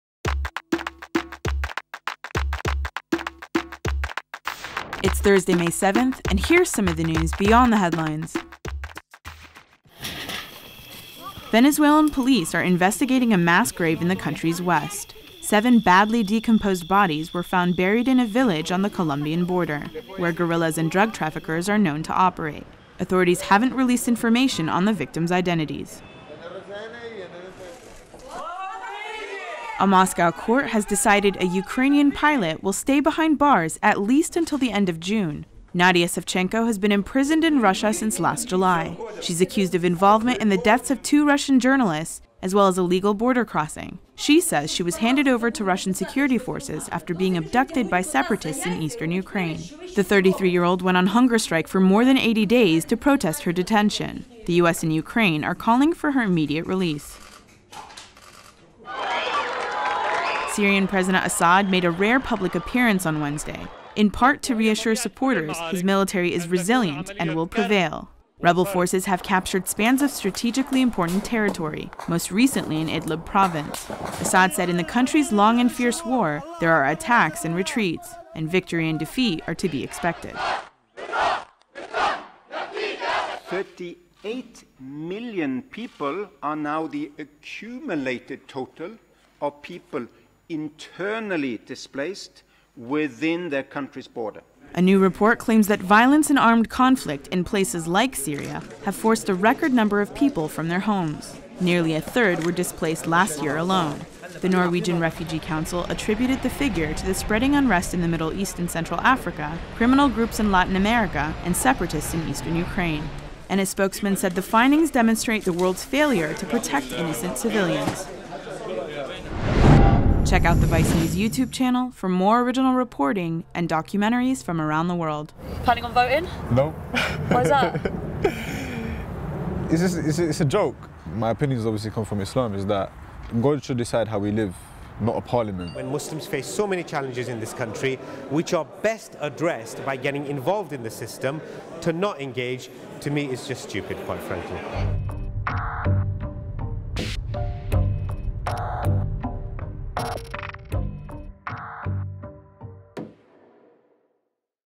VICE News Daily: Record Number of People Displaced by Conflict
The VICE News Capsule is a news roundup that looks beyond the headlines. Today: Mass grave discovered on Venezuela-Colombia border, Ukrainian pilot ordered to remain in Russian custody, Syrian president makes rare public appearance, and a new report says a record 38 million people have been forced from their homes.

VENEZUELA
Mass Grave Discovered Near Colombian Border
Police dug up the badly decomposed bodies of six men and a woman, and say there could be others.

RUSSIA
Court Extends Detention of Ukrainian Military Pilot
Nadia Savchenko will remain behind bars at least until June 30. Kiev and Washington are calling for her immediate release.

SYRIA
President Assad Says Military Setbacks are ‘Normal’
The embattled leader made the comments regarding the army's latest territorial losses.

GLOBAL
Report Claims Record Number of Internally Displaced People
​Violence and armed conflict have forced at least 38 million people from their homes, 11 million in 2014 alone.​